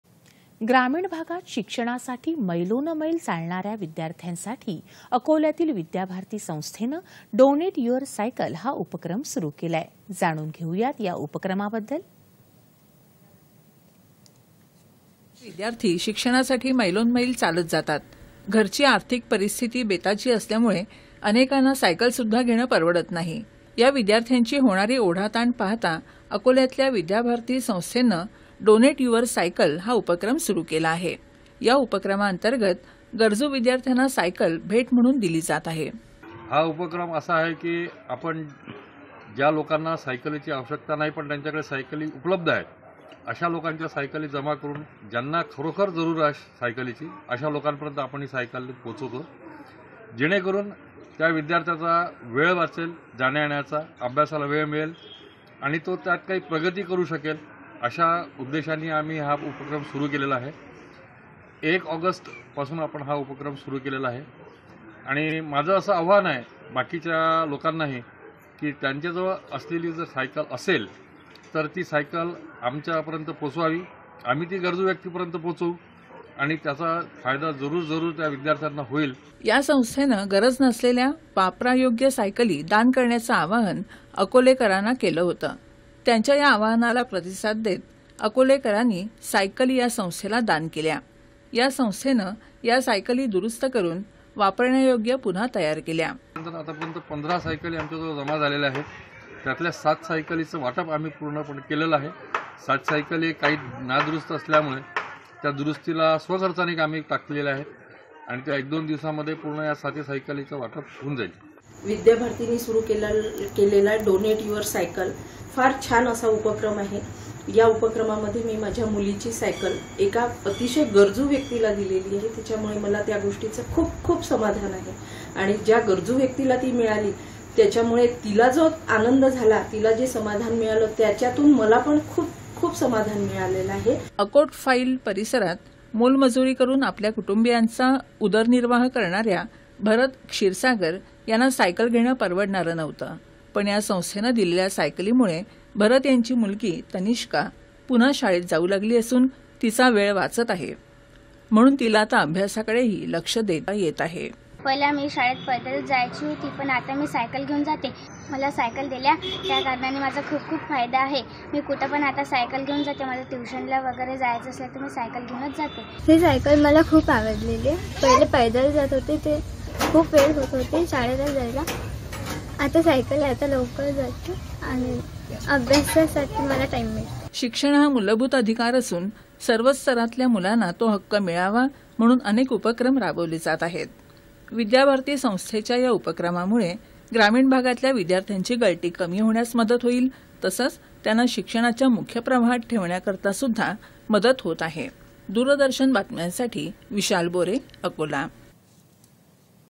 शिक्षणासाठी पायपीट करणाऱ्या ग्रामीण विद्यार्थ्यांसाठी सायकल-दान
दि.३०.०८.२०२३ :
ग्रामीण भागात शिक्षणासाठी मैलोन मैल चालणाऱ्या विद्यार्थ्यांसाठी, अकोल्यातील विद्याभारती संस्थेनं, डोनेट युअर सायकल, हा उपक्रम सुरू केला आहे. जाणून घेऊया या उपक्रमाबाबत.

8.30am | 11.00am | 1.00pm | 4.30pm | 7.00pm | 9.30pm